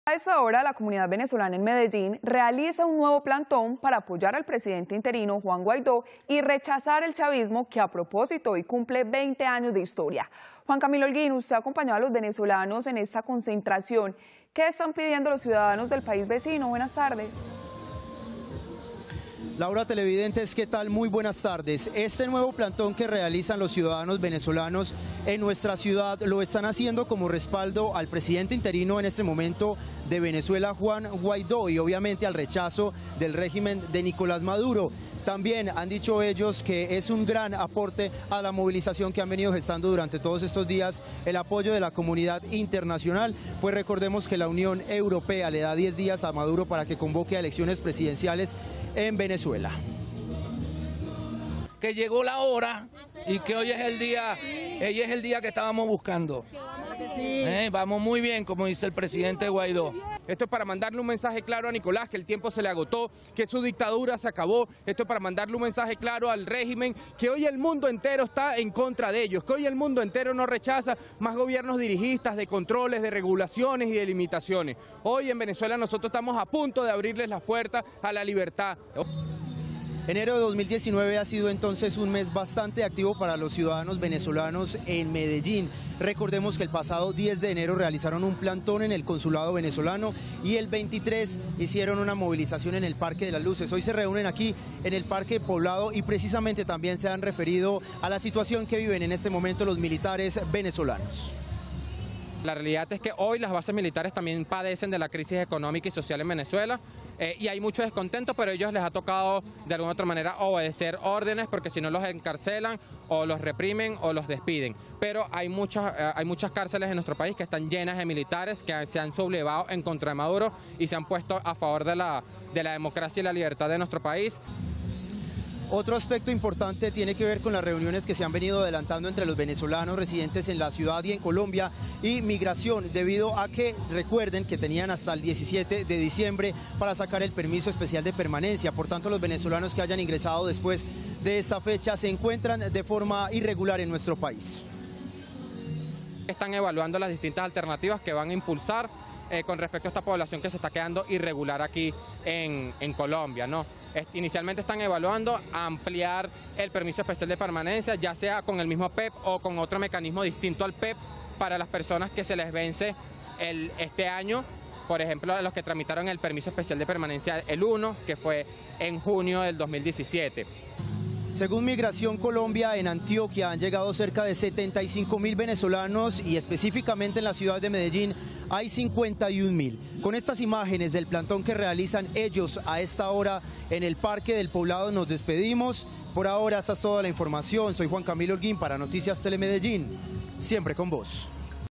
Venezolanos en Medellín realizan un nuevo plantón para apoyar a Juan Guaidó - Telemedellín
La comunidad venezolana en Medellín realizó un nuevo plantón para apoyar al presidente interino Juan Guaidó y rechazar el chavismo que a propósito hoy cumple 20 años de historia.